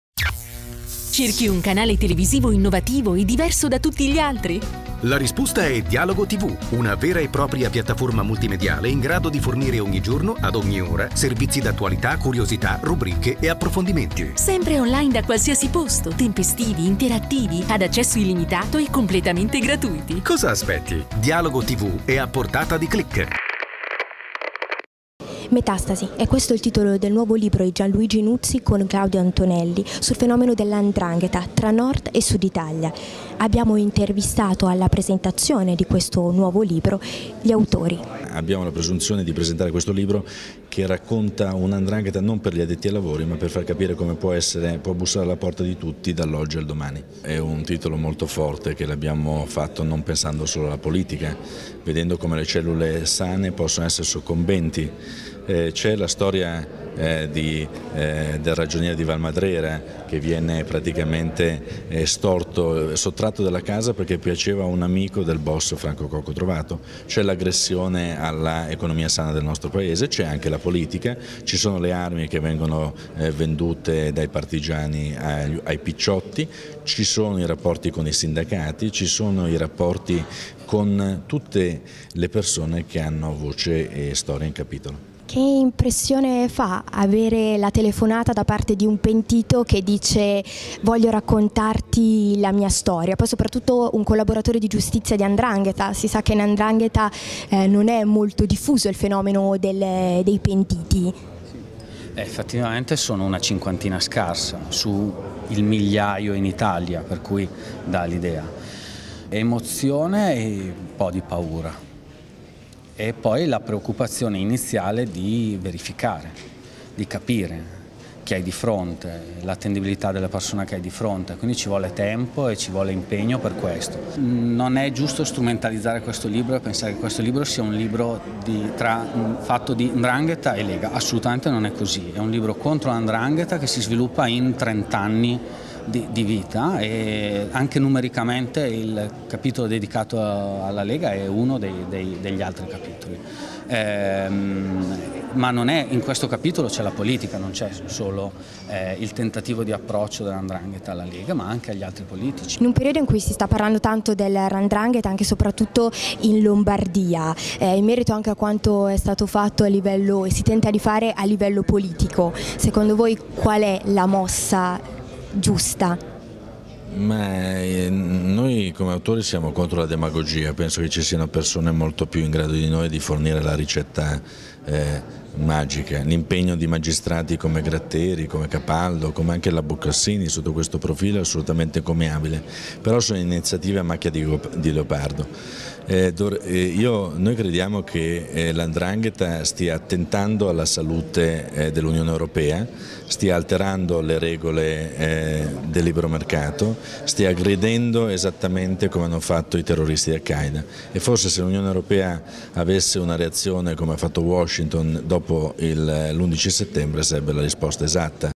Un pentito racconta la 'Metastasi' della 'ndragheta.
La 'ndrangheta come un cancro tra Nord e Sud Italia, ricostruita a partire dalle confessioni di un pentito. Dopo il successo di Vaticano Spa, Gianluigi Nuzzi, insieme al collega di 'Libero', Claudio Antonelli torna con un altro libro destinato a far molto parlare di sé: 'Metastasi', edito da Chiarelettere. Un viaggio nel cuore dell'organizzazione raccontata da un collaboratore di giustizia dove politica, imprenditoria e moda (si parla anche della misteriosa morte di Gianni Versace) sono cellule indiscriminatamente avvelenate da quel tumore subdolo che si chiama 'ndrangheta. Abbiamo incontrato gli autori in occasione della presentazione ufficiale del 2 dicembre.